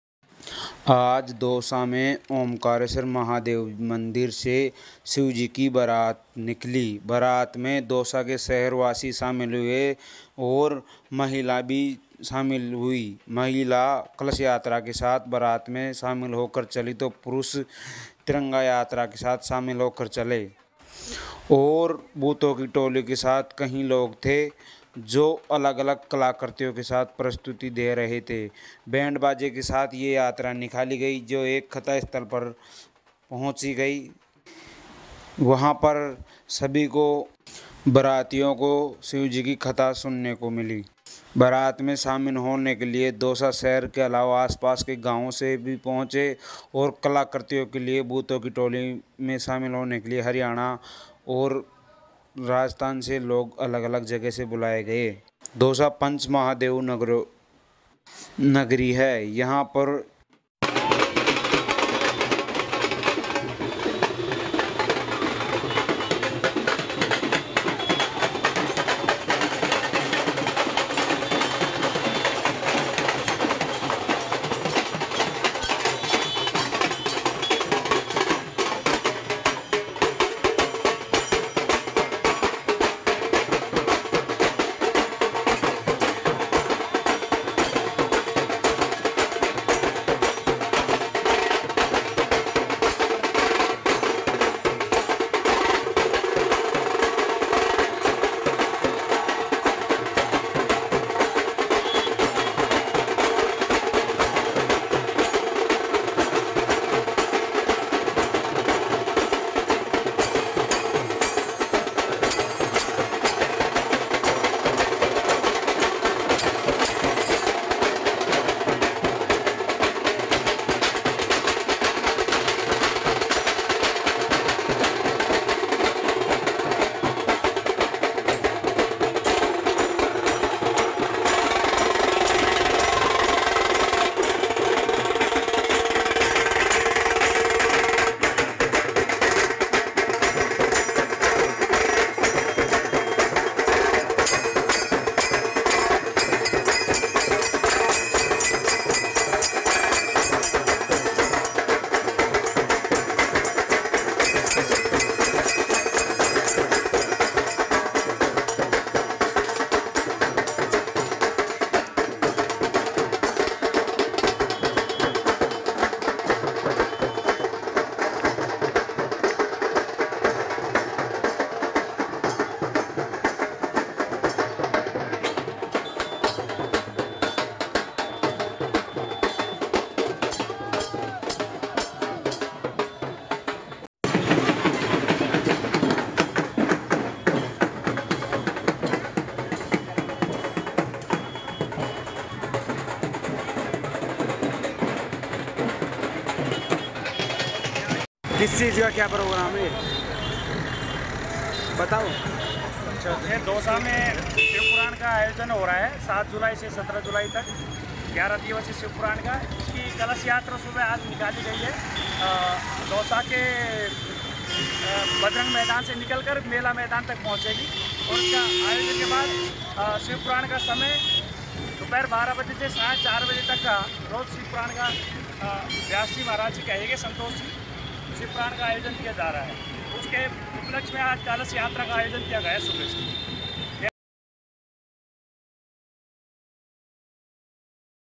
दौसा में कावड़ा यात्रा और भूतों की टोली के साथ निकली शिवजी की बारात, कलाकारों ने भजनों की धुन पर दी म
कालूराम जाट/दौसा. जिला मुख्यालय के बारादरी स्थित सीताराम मंदिर में शुक्रवार को शिव महापुराण कथा की स्थापना हुई. इसे लेकर कलश यात्रा के साथ शिव बारात का आयोजन किया गया. सुबह करीब 10 बजे खादी भंडार रोड स्थित ओम्कारेश्वर महादेव मंदिर से भगवान शंकर की बारात रवाना होकर शहर के मानगंज, रेलवे स्टेशन, गांधी तिराहा, लालसोट रोड, आनंद शर्मा स्कूल, मेला मैदान होते हुए बारादरी मंदिर परिसर पहुंची.

भोले की बारात में पुरुषों के साथ सैकड़ों महिलाएं कलश लेकर चली तो तिरंगा यात्रा और कावड़ यात्रा में भी आस्था का सैलाब देखने को मिला. आयोजन समिति के संयोजक ने बताया कि बारादरी मंदिर में 7 से 17 जुलाई तक रुद्राभिषेक और शिव महापुराण कथा के साथ सवा करोड़ महामृत्युंजय जाप और सवा लाख पार्थिव शिवलिंग निर्माण रूद्र महायज्ञ का आयोजन किया जा रहा है. दौसा एक देवनगरी है क्योंकि यहां पर पंच महादेव है यहां पर शिवजी को लेकर सभी तरह के काम शहरवासी करते हैं कलश यात्रा, पूजा पाठ, कथा आदि का प्रोग्राम यहां पर होते रहते हैं.

कलाकारों ने भजनों की धुन पर दी मनमोहक प्रस्तुतियां

बरात में भगवान भोलेनाथ दूल्हे के रूप में नंदी पर सवार होकर चले तो वहीं, शहरवासी बाराती के रूप में बैंड बाजे के साथ नाचते-गाते हुए शामिल हुए. भोले की बारात में अलवर और हरियाणा से आए कलाकारों द्वारा अघोरी भूतों की झांकी, अजमेर के कलाकारों ने ढोल मंजीरे के साथ भजनों की धुन पर मनमोहक प्रस्तुतियां दी. इन सभी प्रस्तुतियों में और बारात में शामिल होने के लिए शहर के अलावा आसपास के गांवों से भी लोग पहुंचे तो कुछ लोग ने प्रस्तुति दी और कुछ बारात में भी शामिल हुए.